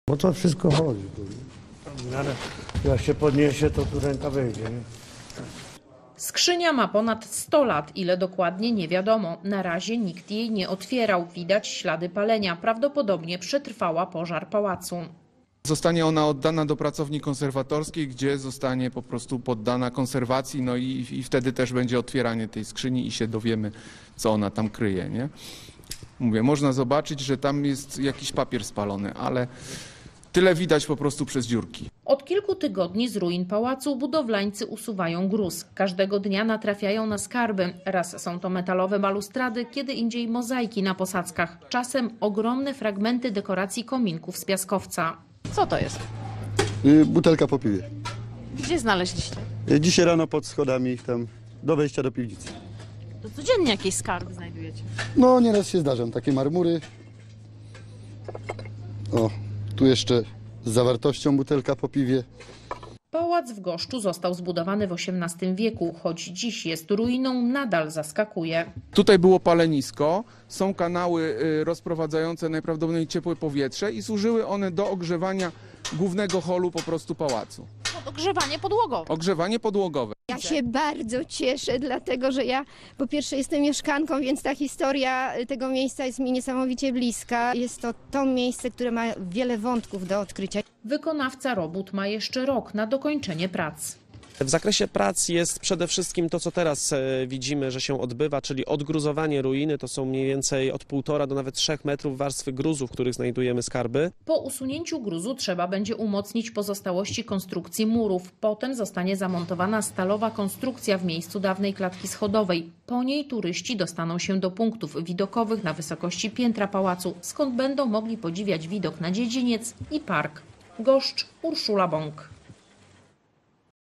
🥇Pod gruzami pałacu odkryto zabytkową skrzynię. Czy zawiera skarb? Wykopki 2022 Poszukiwacze skarbów
Któż by nie chciał znaleźć takiej skrzyni?